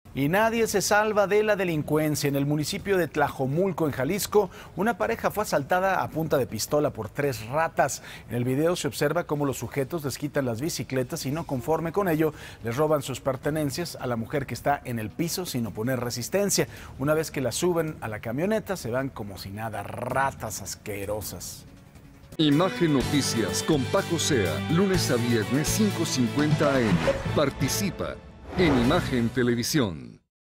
Asaltan a pareja en Jalisco y les roban sus bicicletas | Noticias con Francisco Zea
¡Nadie se salva de la delincuencia! Asaltan a una pareja en Jalisco y les quitan sus bicicletas.